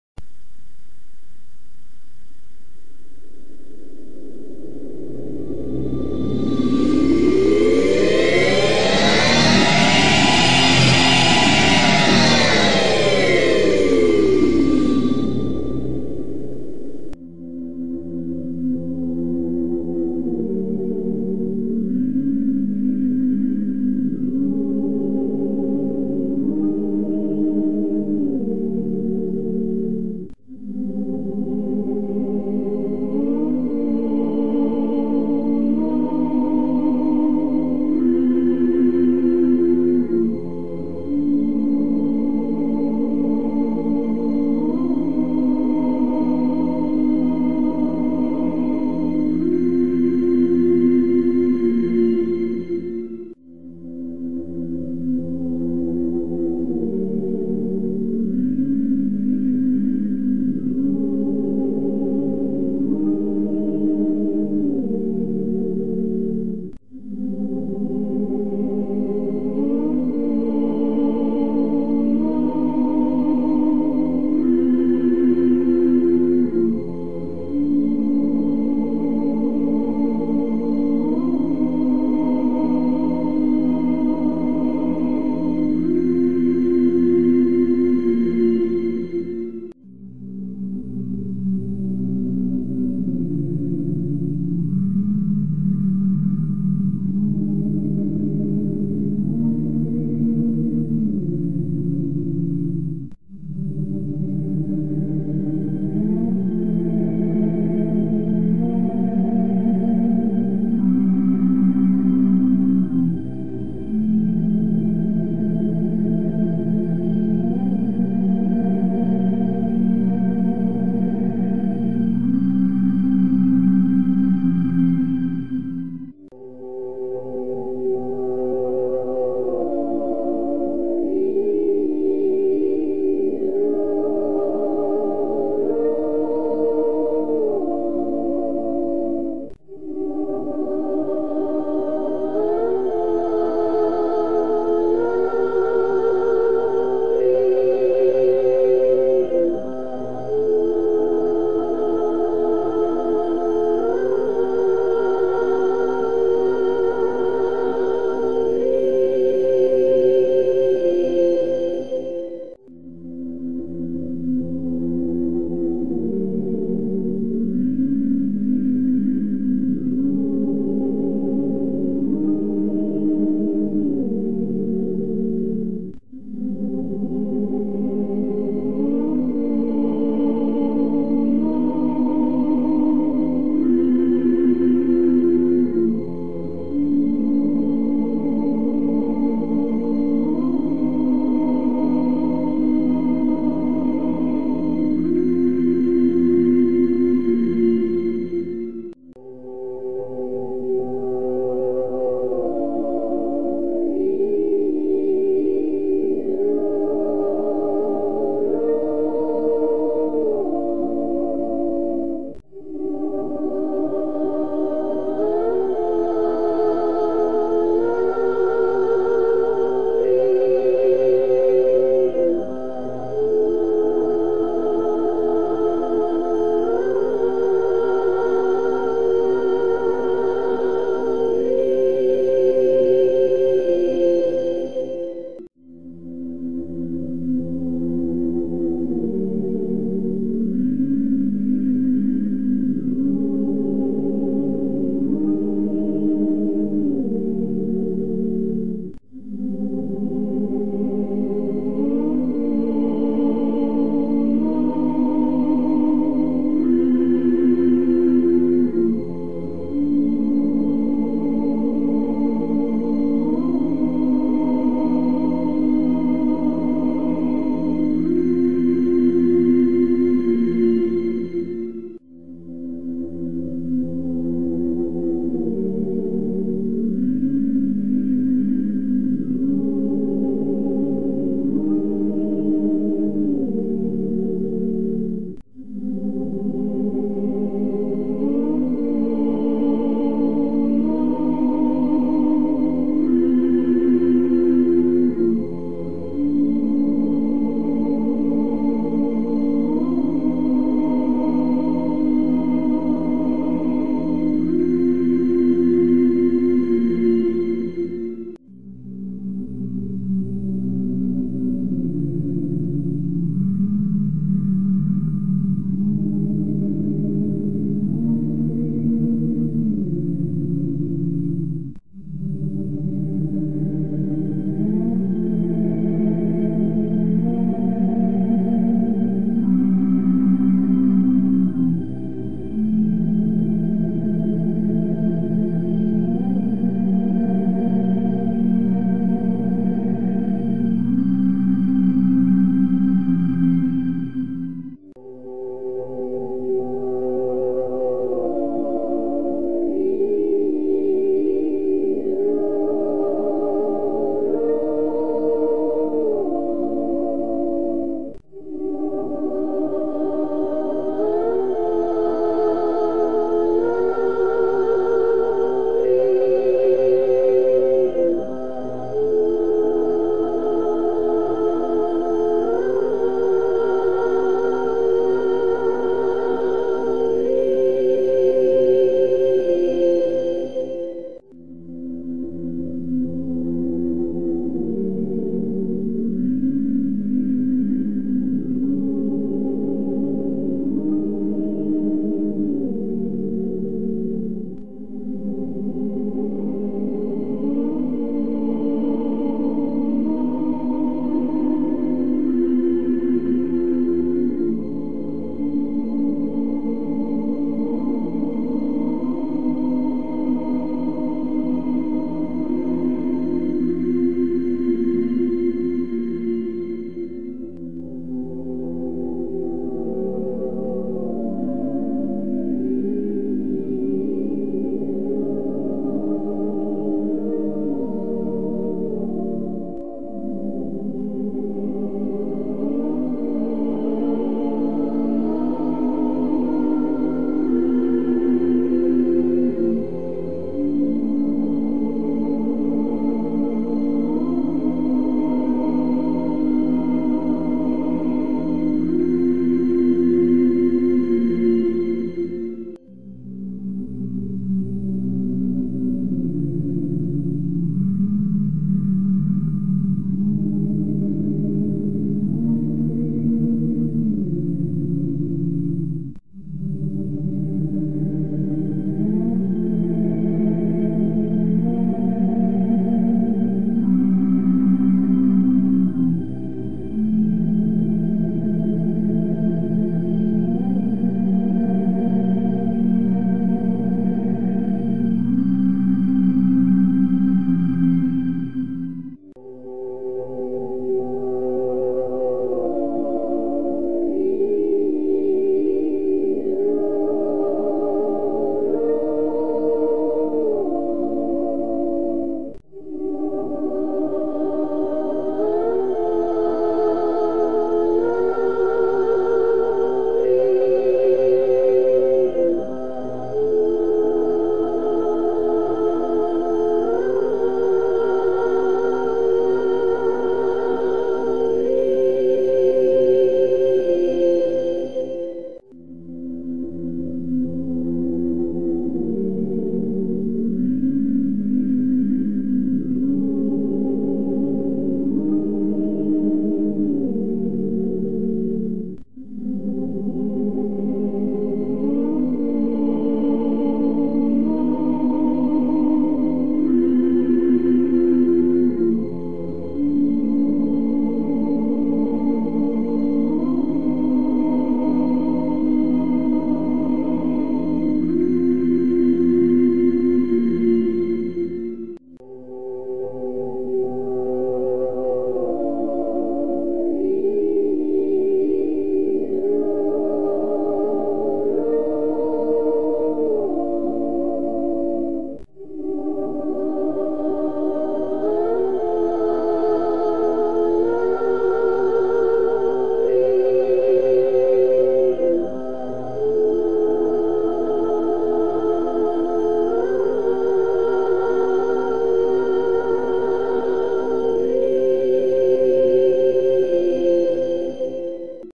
First Photo's Ever taken in Date order
Our First Visually recorded images from 1839 in date order to 1845.
There are no words or meanings to the music They were recorded tones from a keyboard
The music was not meant to distract the viewer from the images but form a neutral background.
Therefore an original combination of sounds that can not be like their music or our music or ever was needed